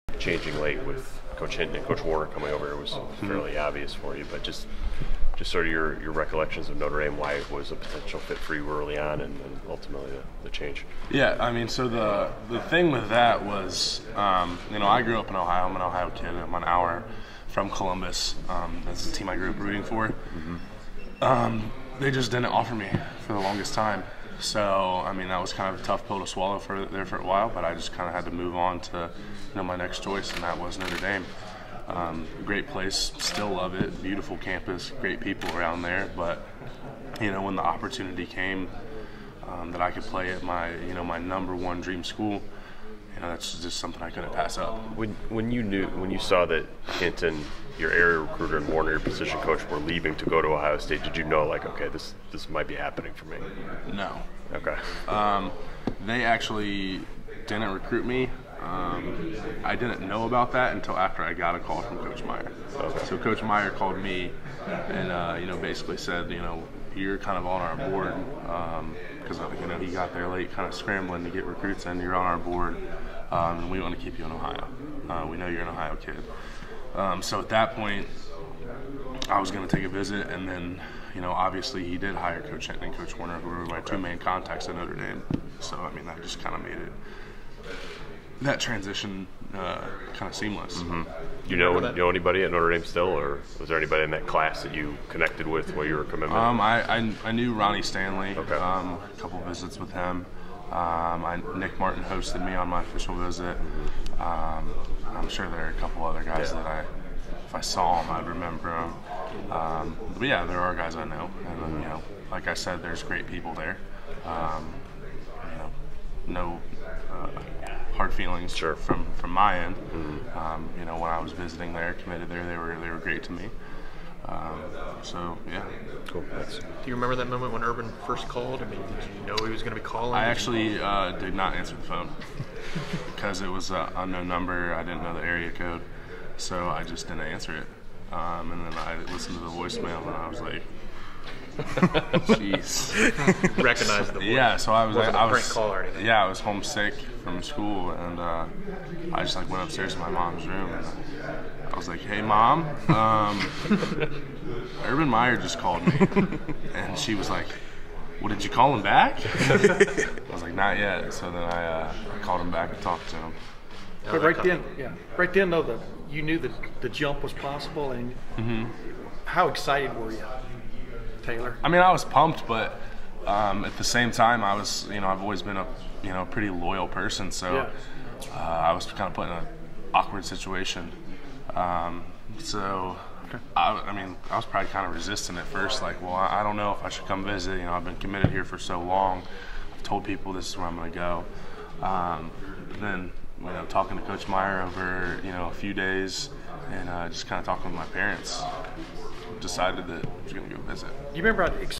Taylor Decker :: December 29th, 2015 - ELEVENWARRIORS.COM
Ohio State's Taylor Decker talks to the media about the Buckeyes' upcoming game with Notre Dame.